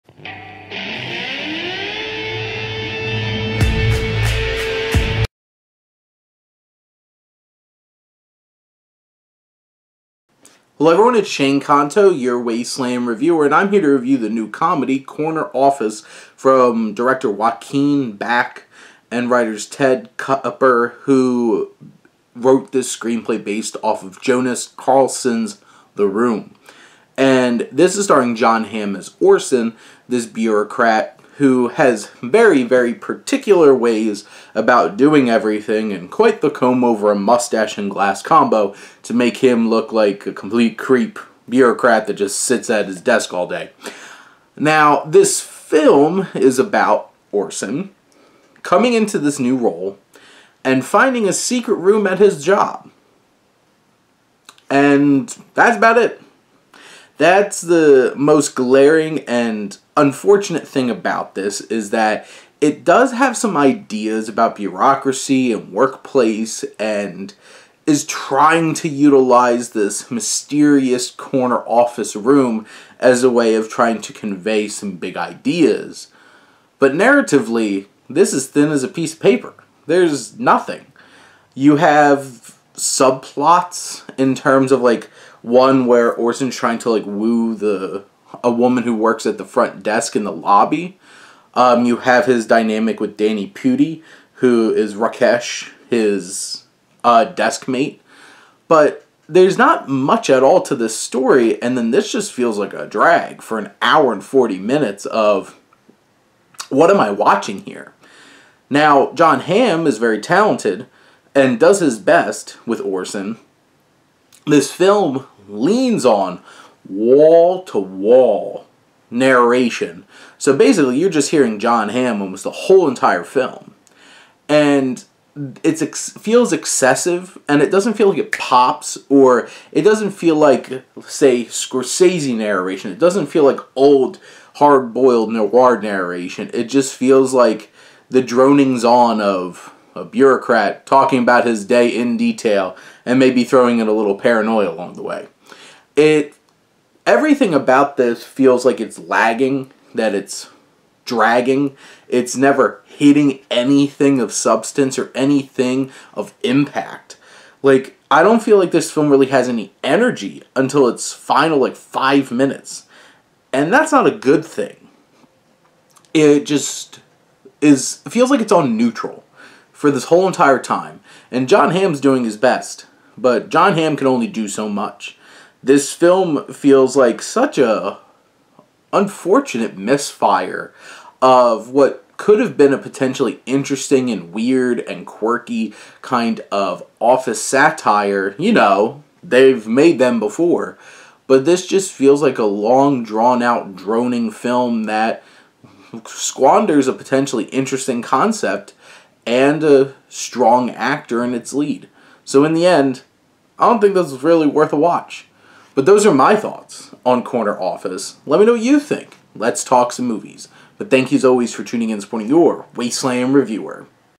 Corner Office (2023) - Wasteland Film Review
My Wasteland Review of Corner Office
Theaters & VOD August 4th
Synopsis: Orson, a compulsive bureaucrat who discovers a secret room.